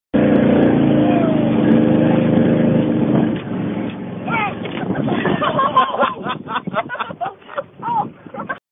Laundry Tub FAIL!!!
This is one of my friends in a laundry tub with a rope tied to it and a golf cart. The tub hits an ant bed and breaks, sending him flying.